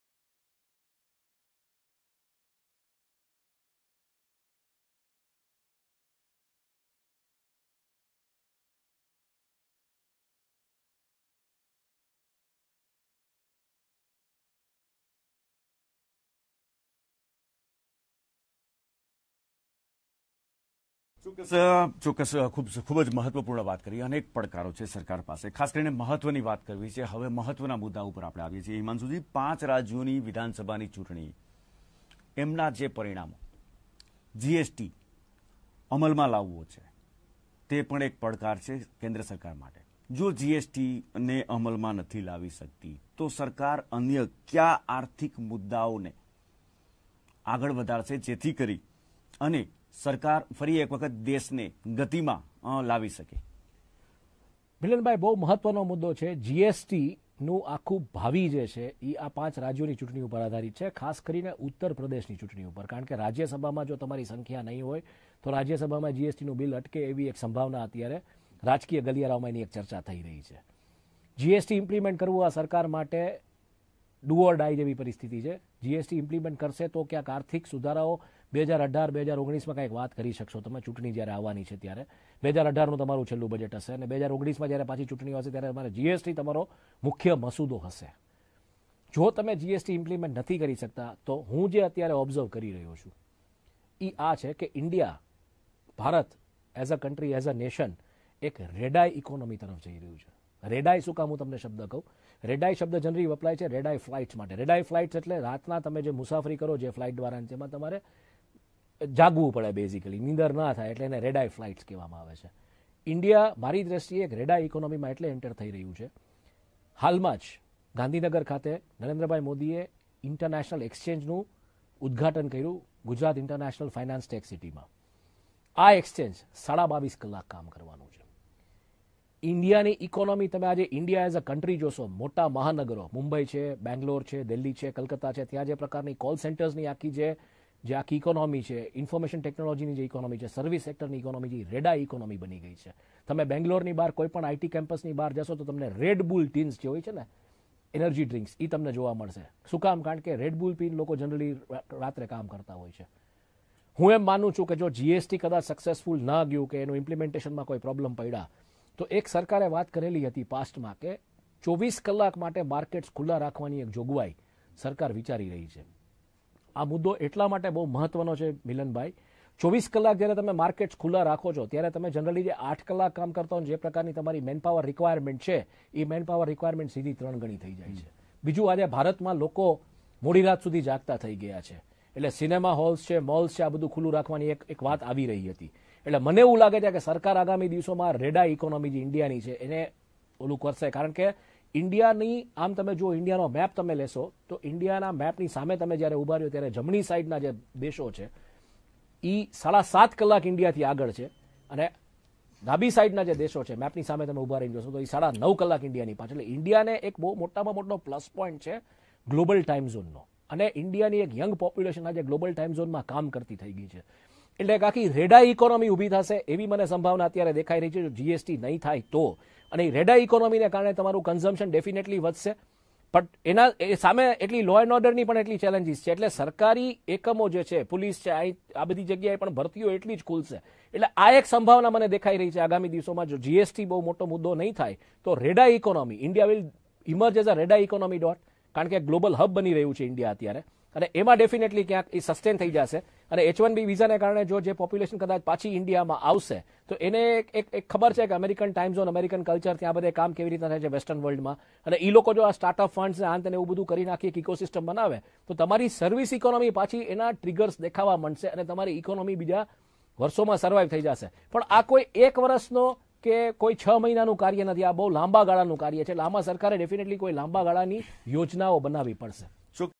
On RED-EYE ECONOMY (24x7 OPEN MARKETPLACES)
Had forecast of RED-EYE ECONOMY (24x7 OPEN MARKETS) well 737 days before it got implemented, while discussing POST BUDGET ANALYSIS on 2nd February, 2017 - Thursday.

सल २०१७ के बजेट के बाद दिनांक २ फरवरी २०१७के गुरूवार के दिन की गई टेलीविज़न चर्चा में रेड-आई इकोनॉमी  (२४x७ खुल्ले बाज़ारो वाली अर्थव्यवस्था) के बारे में ७३७ दिन (२ साल) पहेले की थी भविष्यवाणी

સાલ ૨૦૧૭નાં બજેટ બાદ તારીખ ૨જી ફેબ્રુઆરી ૨૦૧૭ની સાલમાં ગુરુવારે કરેલી ટેલિવિઝન ચર્ચામાં રેડ આઈ ઈકોનોમી (૨૪x૭ ખુલ્લા બજારો વાળી અર્થવ્યવસ્થા) વિષે આજથી ૭૩૭ દિવસો પહેલાં (એટલે કે ૨ વર્ષ પહેલાં) ભવિષ્યવાણી કરેલી હતી